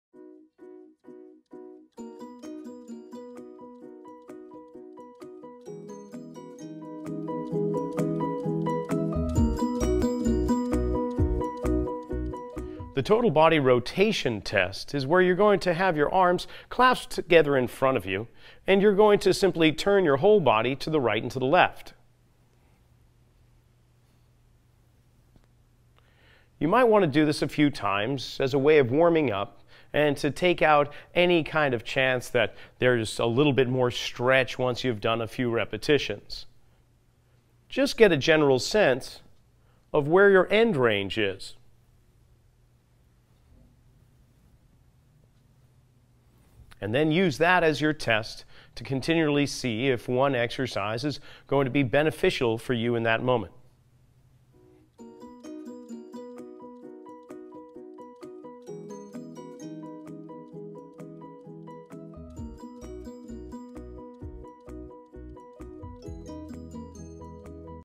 Return to Center ANS: Total Body Rotation Test
This video is to support concepts in the book, Return to Center (2019). The video will guide the viewer through a means of assessing how the Autonomic Nervous System responds to any exercise or movement.The exercises in this channel are produced as a means of assisting people with exercise guidance, selection, and execution.